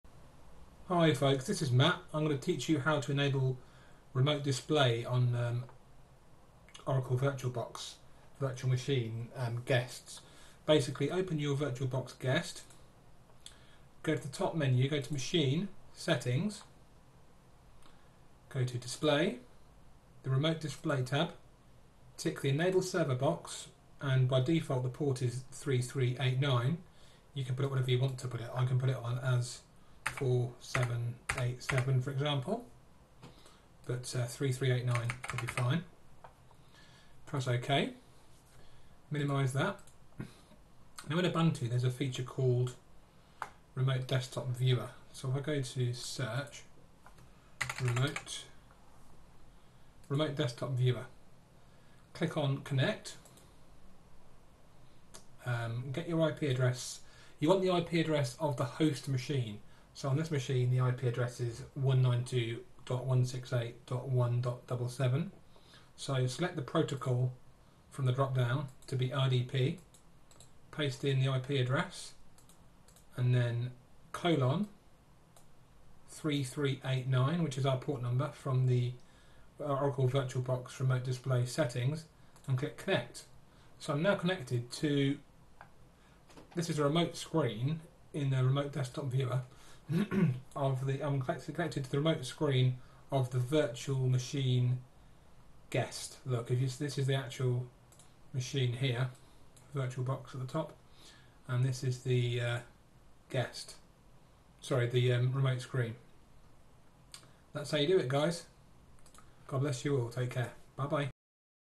Oracle VirtualBox: Setting up and viewing the "Remote Display" of a guest machine
I show you how to enable the "Remote Display" feature, in your Oracle VirtualBox guest settings, and then show you how to view the guest via RDP, on Ubuntu's "Remote Desktop Viewer" client. Simple!